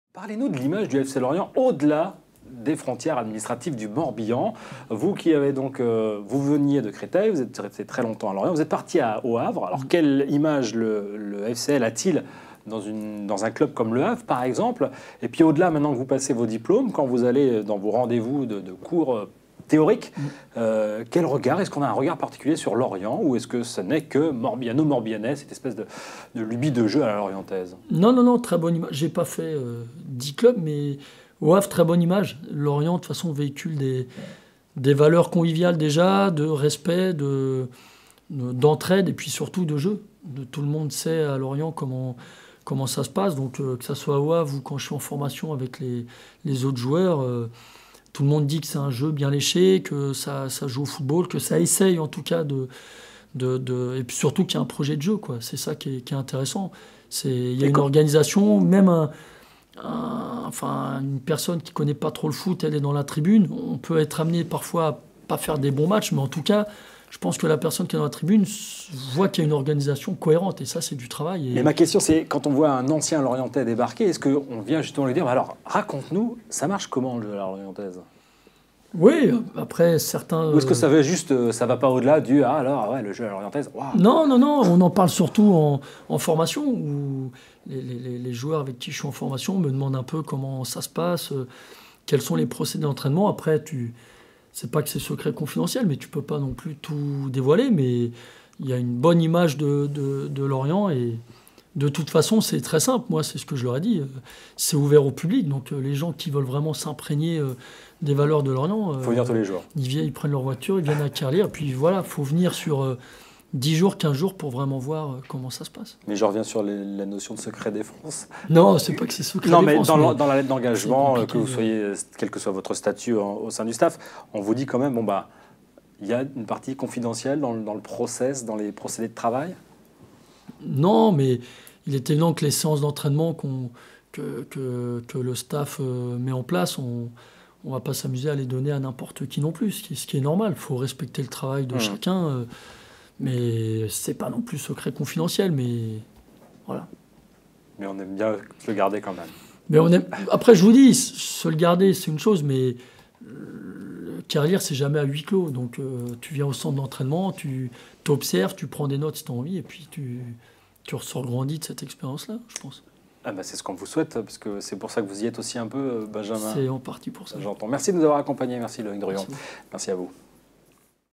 "On ne va pas s'amuser à les donner à n'importe qui non plus" B.Genton
"Quels sont les procédés d'entraînement ? Ce n'est pas que c'est un secret confidentiel, mais tu ne peux pas non plus tout dévoiler."
Revenu au FC Lorient pour intégrer le staff technique dans le cadre de sa formation au DEPF (diplôme d'entraineur), Benjamin Genton, ancien défenseur central du FC Lorient (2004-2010), sait combien l'image du FCL s'est forgée sur son identité de jeu. Il aime à rappeler que les séances d'entraînement sont "ouvertes au public. Les gens qui veulent s'imprégner des valeurs de Lorient (n'ont qu'à) venir, 10 ou 15 jours, pour vraiment voir".